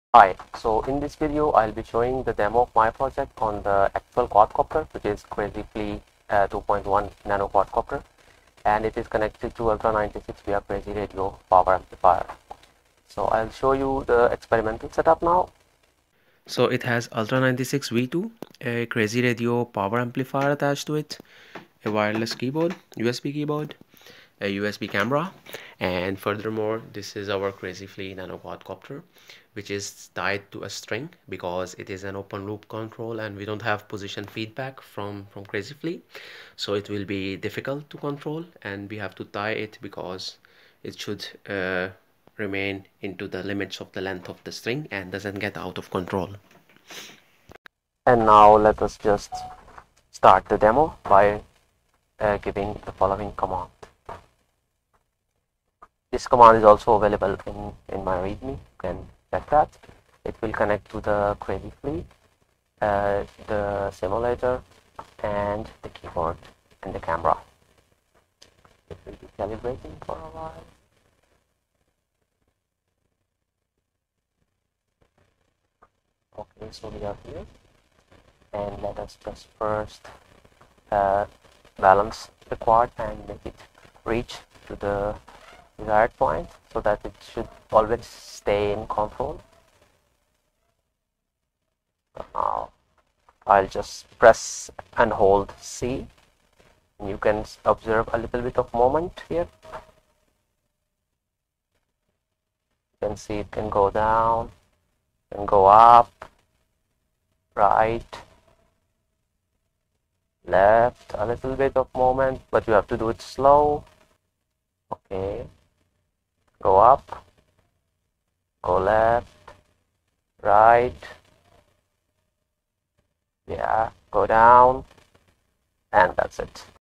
Quadcopter control using Deep Reinforcement Learning and Hand Gestures on Ultra96 [Crazyflie demo]
The agent is trained using twin delayed deep deterministic policy gradient, rank based prioritized experience replay and feedback. The inference is performed on ultra96, utilizing Xilinx's Vitis-AI inference stack and DPU-PYNQ. The demo is shown with crazyflie 2.1 nano quadcopter.

PS: Sorry for the poor audio.